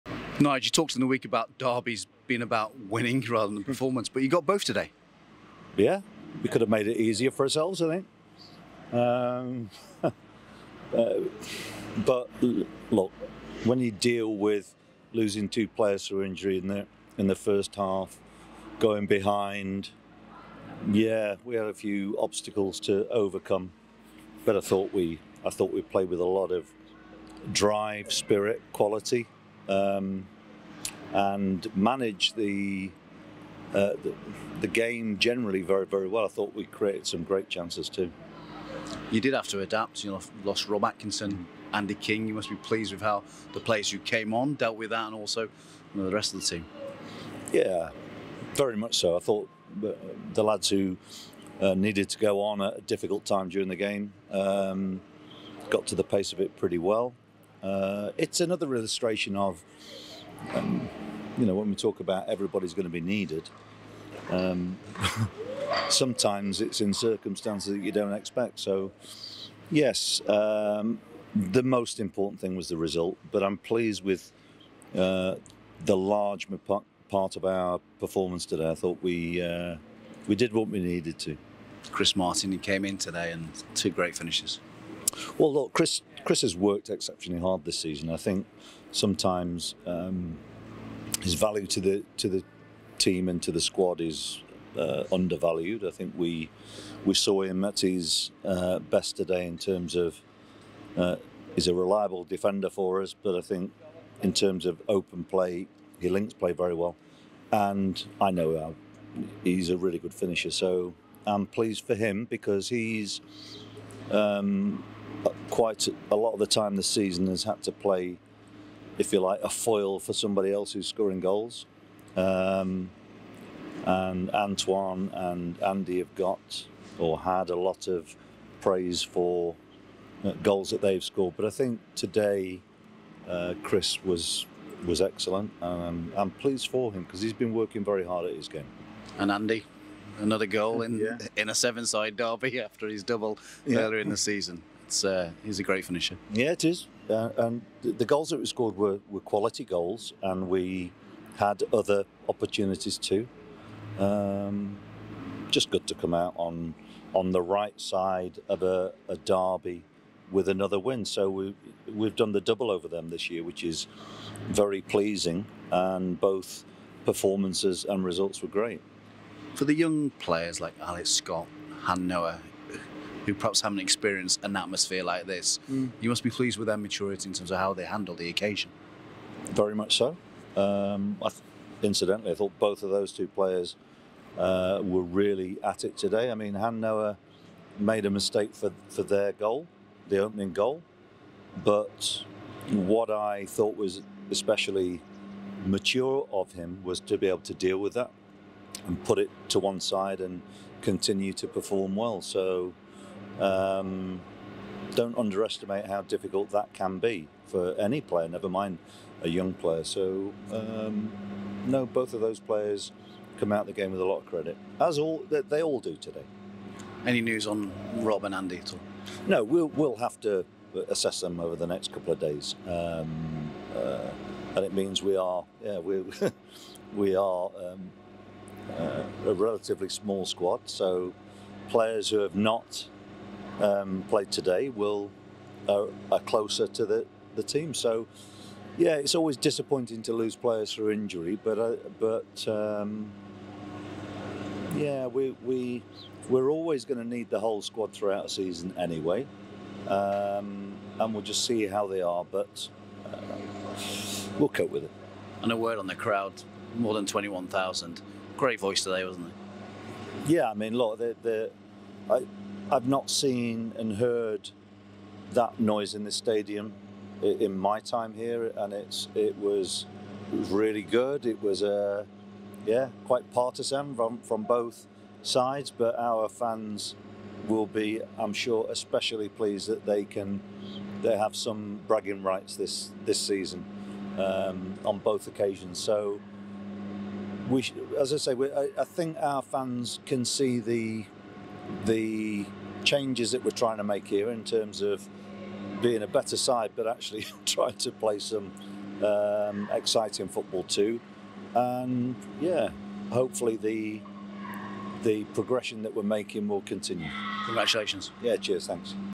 Pearson pleased with ‘spirit, drive and quality’
Manager Nigel Pearson praised City’s ‘spirit, drive and quality’ after the Robins completed the Severnside double over Cardiff City.